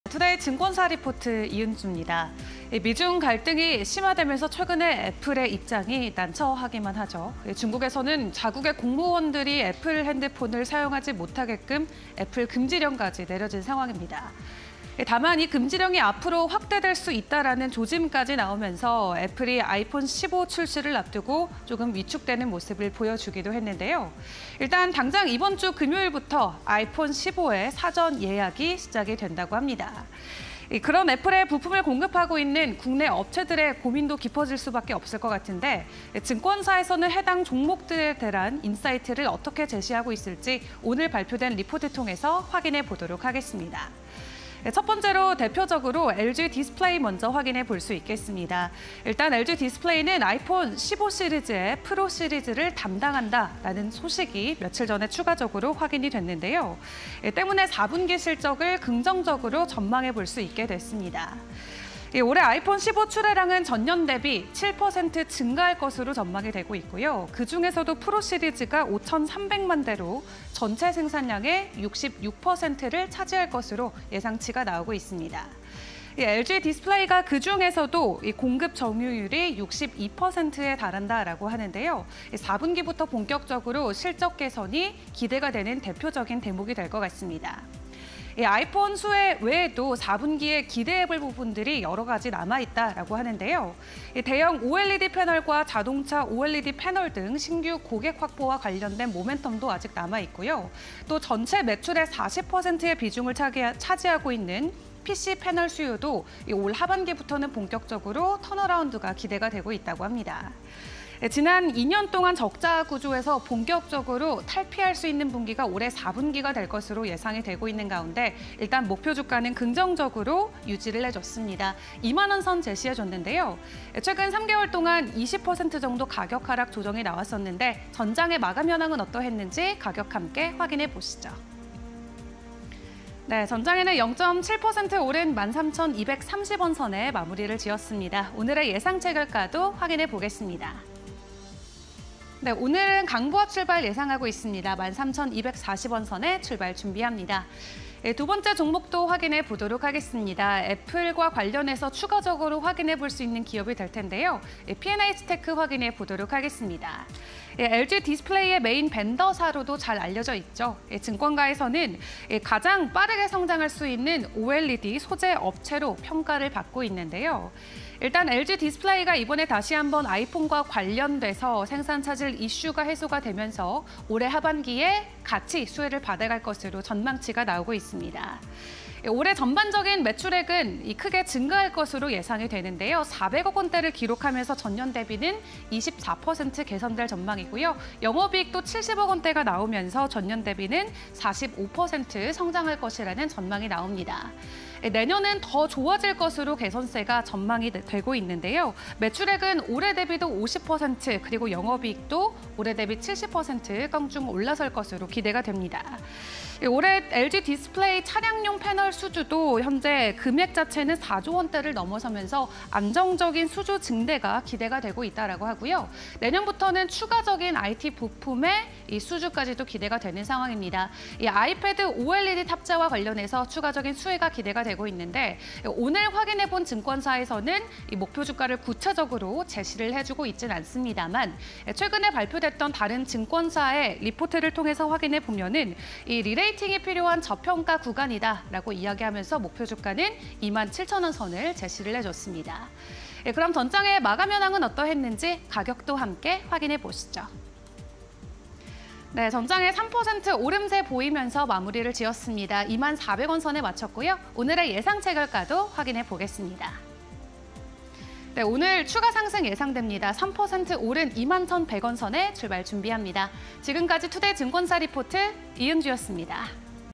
LG디스플레이… 4분기 모멘텀 집중 / 피엔에이치테크… 낙폭과대, 하반기 최대 실적 전망_Today 증권사 리포트 (20230913)
LG디스플레이(034220): 4분기 모멘텀 집중
- 아이폰15 프로 시리즈 OLED 점유율 62% 전망
- 대형 OLED 패널· 자동차 OLED 패널 등 신규 고객 확보

피엔에이치테크(239890): 낙폭과대, 하반기 최대 실적 전망
- 고객사 생산 차질 이슈 해소, 모바일 OLED 매출 확대 예상
- 2023년 매출액 437억원, 영업이익 70억원 전망